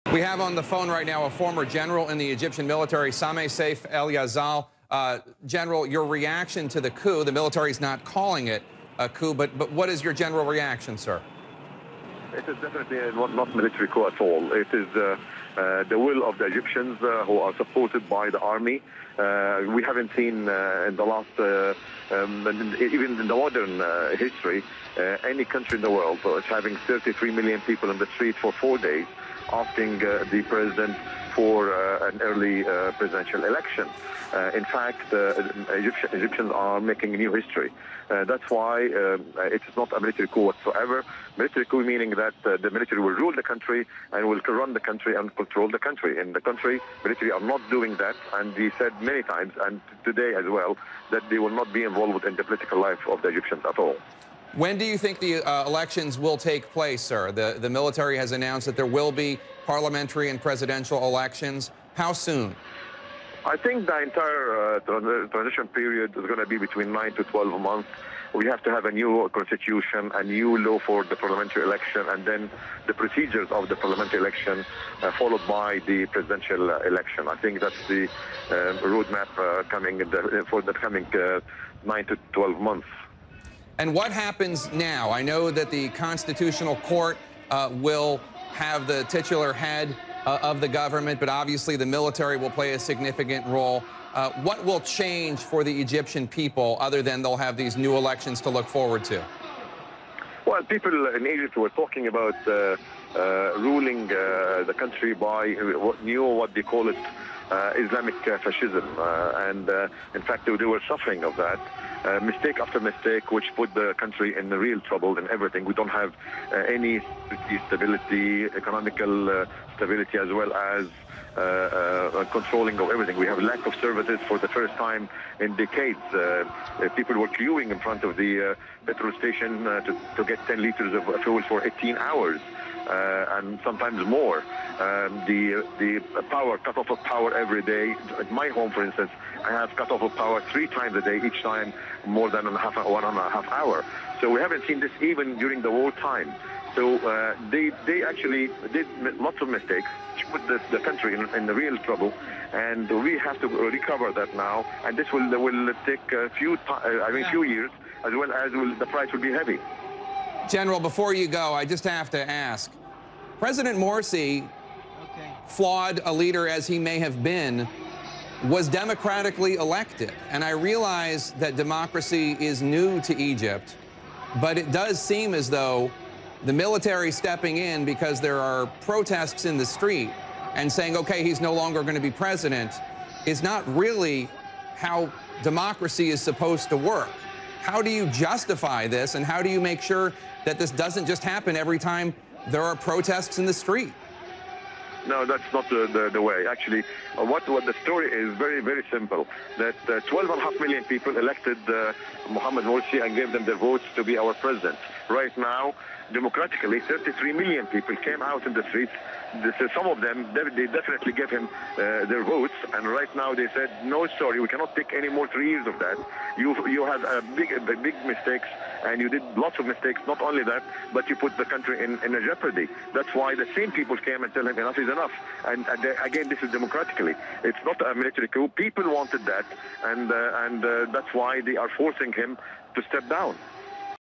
Egyptian general: This isn't a military coup
Egyptian General Sameh Seif Elyazal talks with CNN's Jake Tapper on the ouster of former Preisdent Mohamed Morsy.